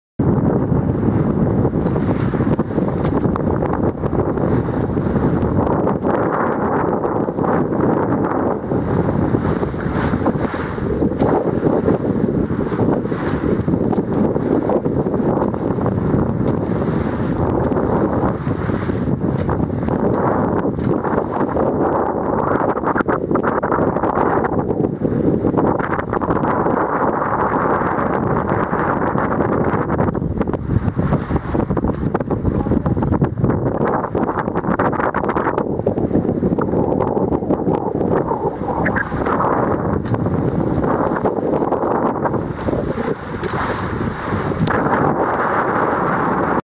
Gdańsk Rzucewo Gdynia na trawersie
Focus 650 płynie z Gdańska do Rzucewa